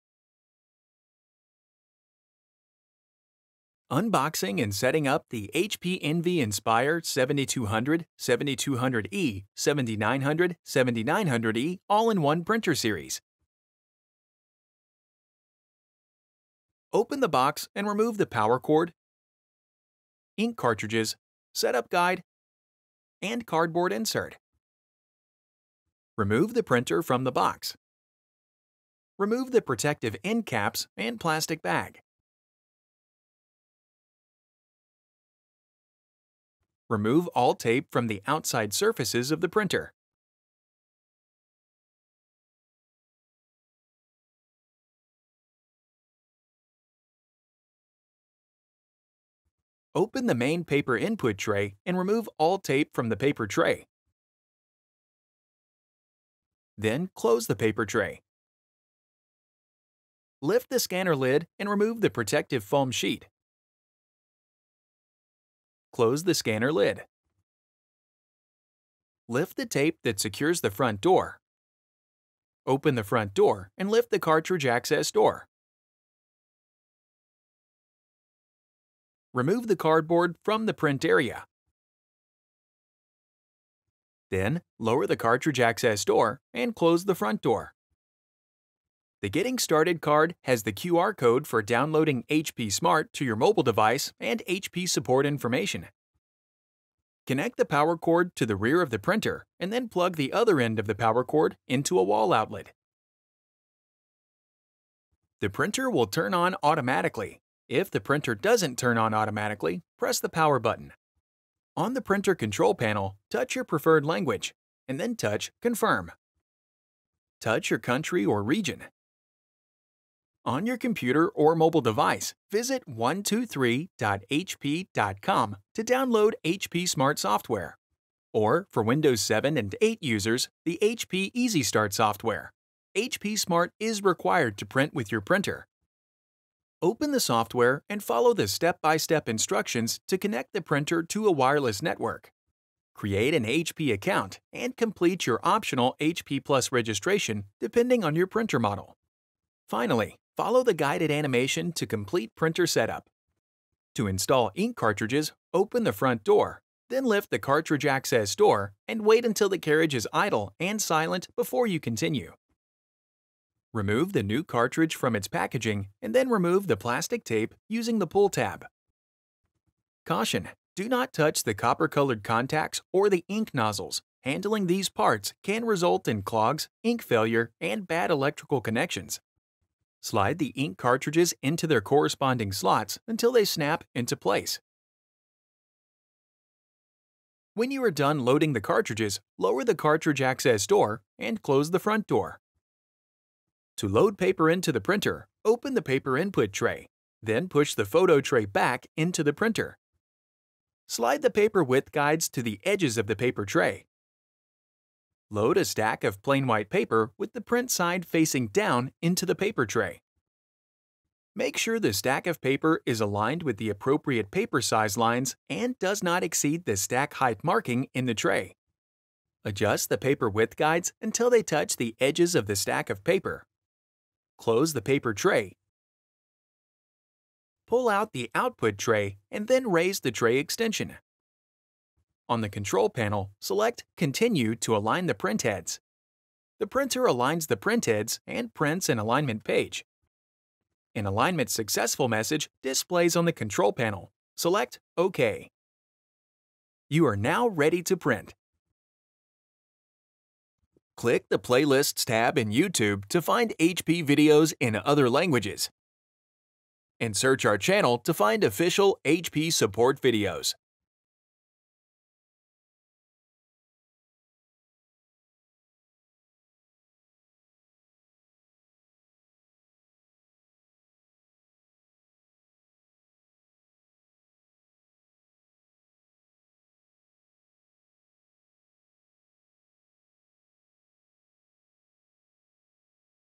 Unbox and Set Up HP ENVY Inspire 7200, 7200e, 7900, 7900e AiO Printers
Learn how to remove the HP ENVY Inspire 7200, 7200e, 7900, 7900e All-in-One Printer Series from its packaging and set up the printer. Also learn how to download the HP Smart printer app and connect wirelessly to your mobile device or personal computer.

Chapters:
00:00 Introduction
00:15 Unbox the Printer
01:28 Download Printer Software
02:32 Install Ink Cartridges
03:12 Load Paper

This video applies to the following models:
HP ENVY Inspire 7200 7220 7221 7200e 7221e 7222e 7224e 7252e 7255e 7258e All-in-One printer
HP ENVY Inspire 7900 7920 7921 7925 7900e 7920e 7921e 7922e 7924e 7925e 7955e 7958e 7964e All-in-One printer

Unbox and Set Up HP ENVY Inspire 7200, 7200e, 7900, 7900e All-in-One Printers | HP Printers | @HPSupport